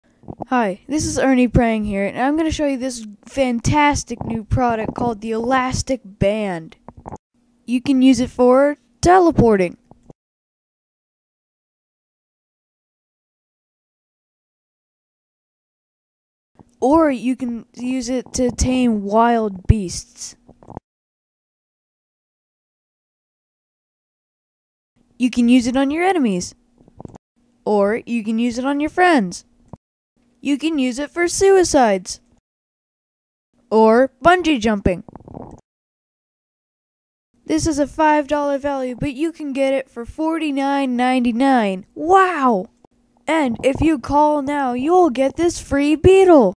The Amazing Elastic Band
Ernie shows you this great new product